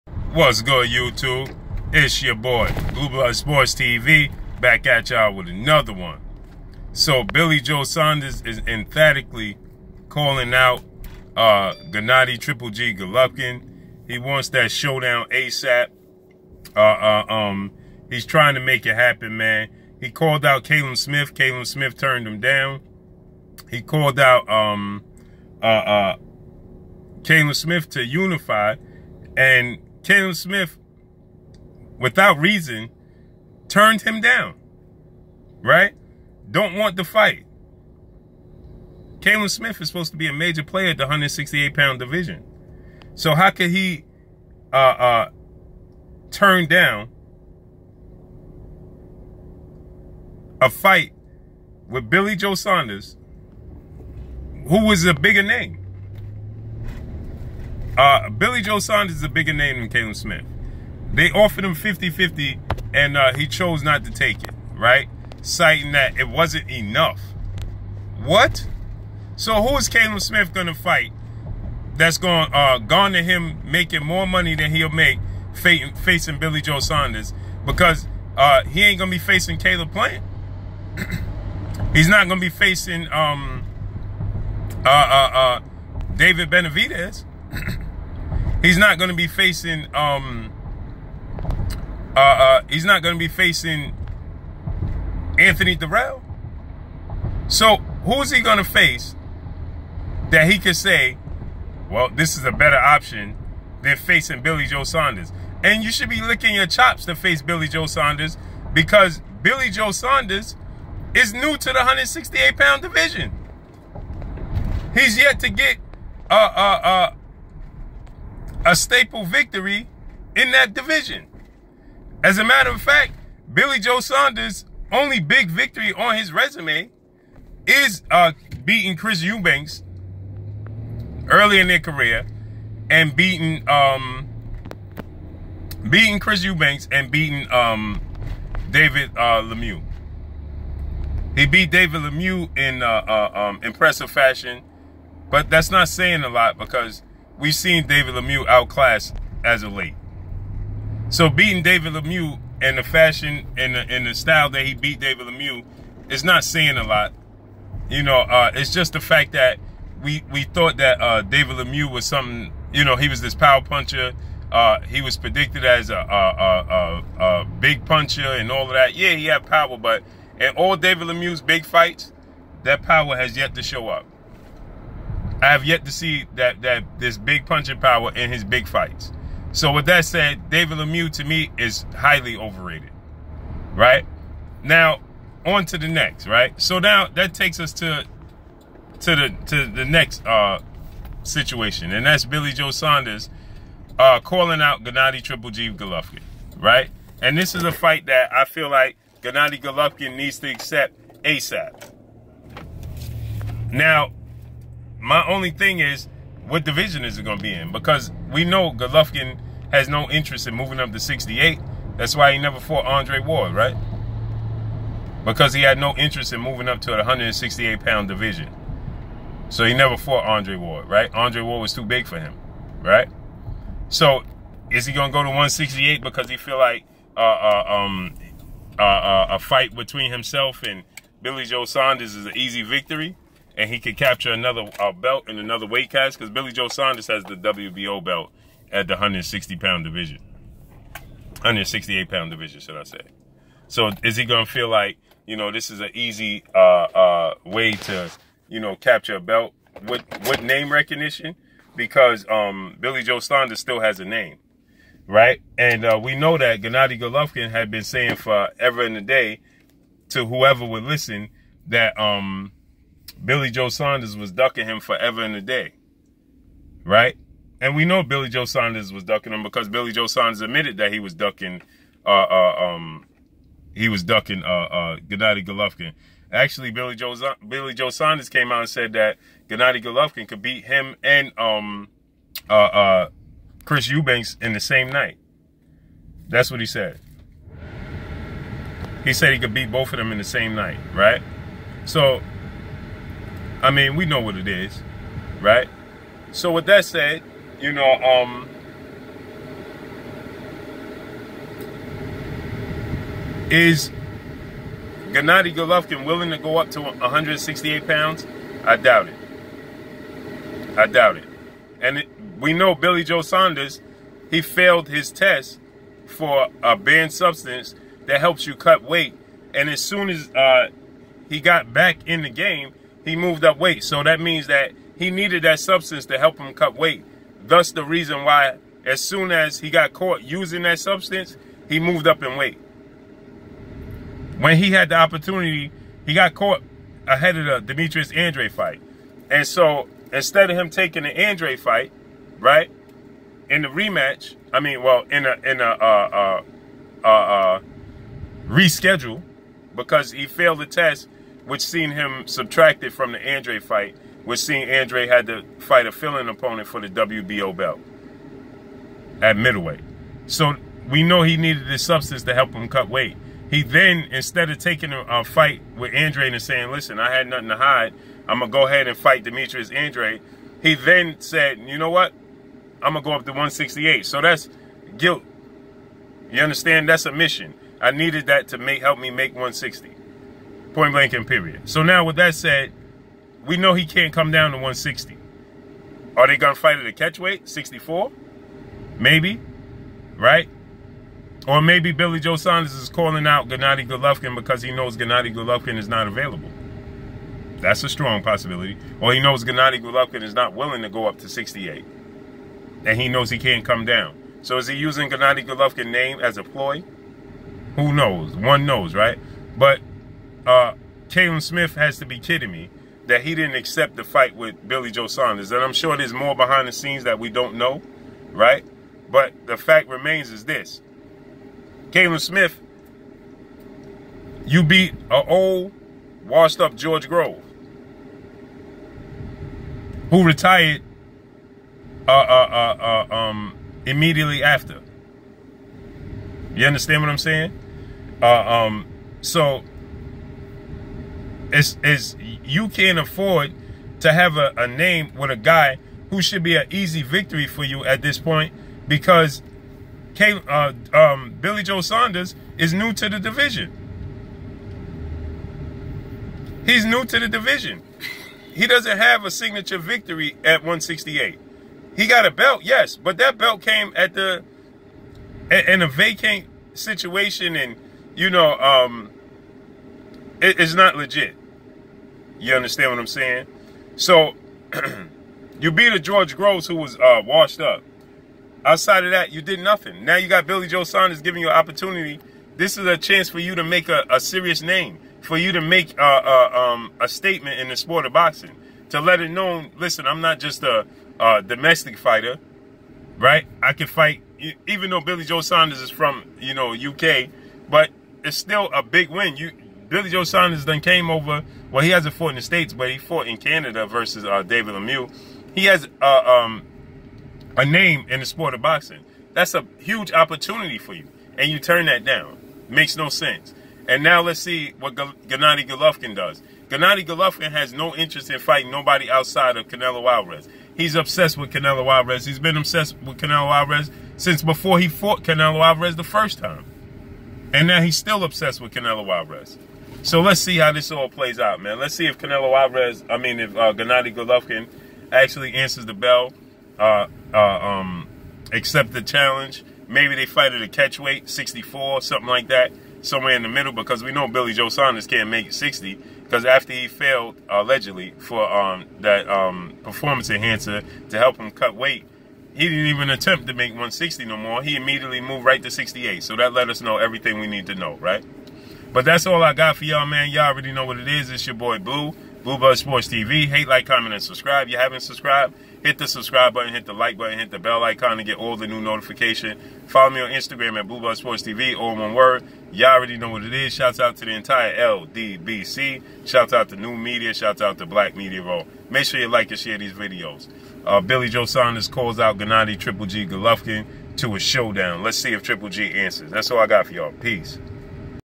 BREAKING NEWS: (WHOA) 😳GENNADY "GGG" GOLOVKIN EMPHATICALLY CALLED OUT BY BILLY JOE SAUNDERS 😳
NEWLY CROWNED WBO SUPER MIDDLEWEIGHT CHAMPION (BILLY JOE SAUNDERS) HAS BEEN ON A CAMPAIGN AS OF LATE, EMPHATICALLY CALLING OUT FORMER UNIFIED MIDDLEWEIGHT CHAMPION (GENNADY "GGG" GOLOVKIN) TO AN IMMEDIATE SHOWDOWN  !!

SOURCE: WORLD BOXING NEWS 24.COM
SOURCE: BLUE BLOOD SPORTS TV